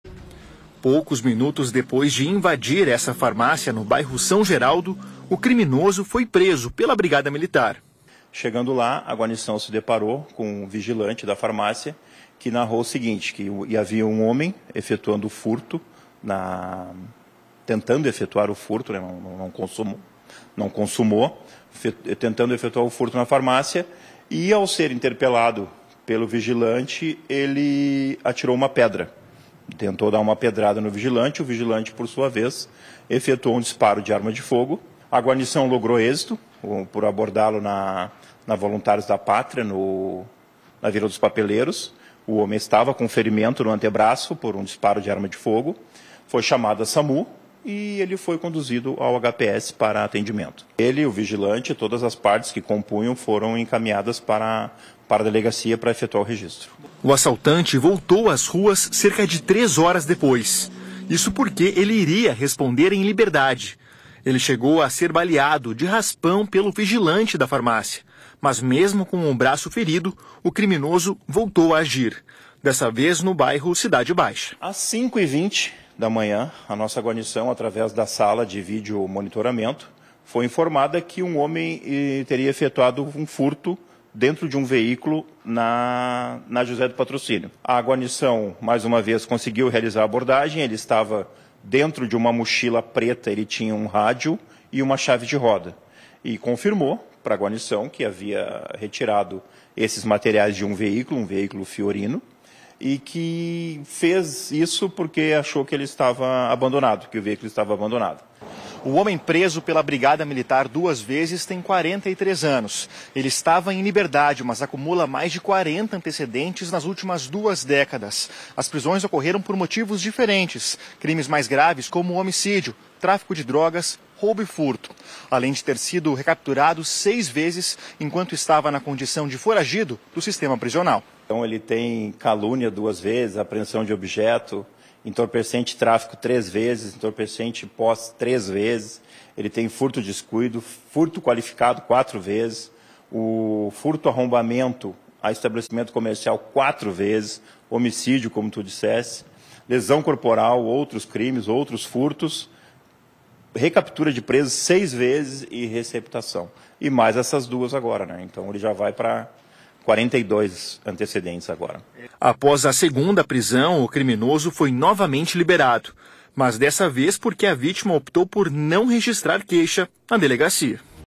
Porto Alegre: criminoso é preso duas vezes na mesma noite furtando
Um homem foi preso duas vezes, na mesma noite, cometendo furtos na zona leste da capital. O tempo entre uma ocorrência e outra foi de poucas horas, enquanto a polícia preenchia o boletim de ocorrência.